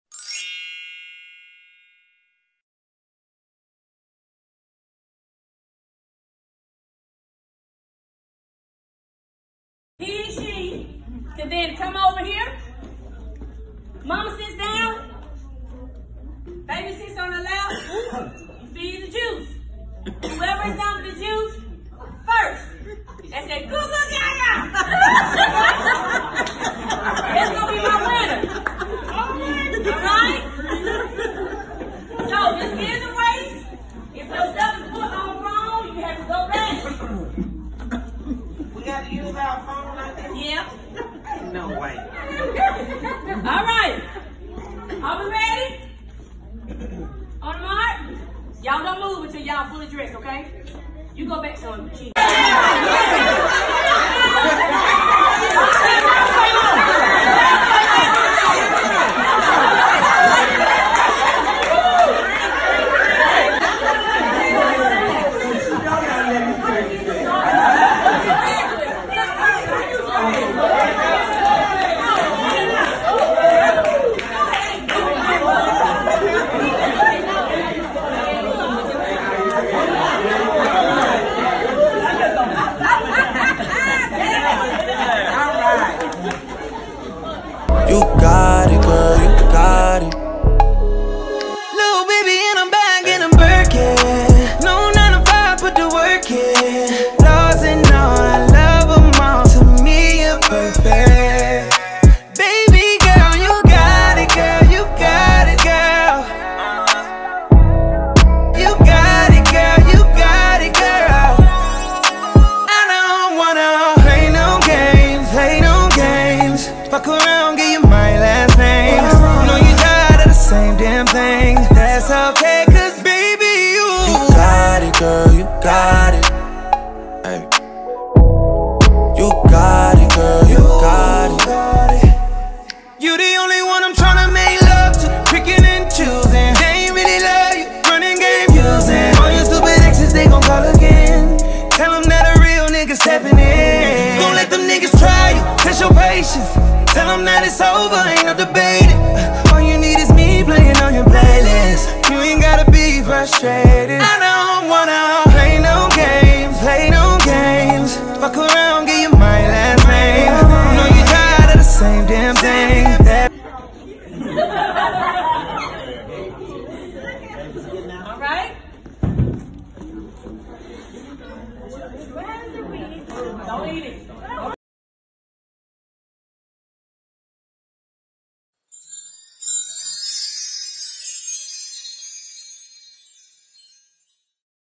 Two Baby Shower Games You’ve Never Seen or Played Before! Safari Baby Shower Decor by EventsbyCCM!
Baby Shower for Princess Payton Rose!

Baby Shower Decorations and Games!

Have you played these games before at a baby shower?

What is your favorite game to play?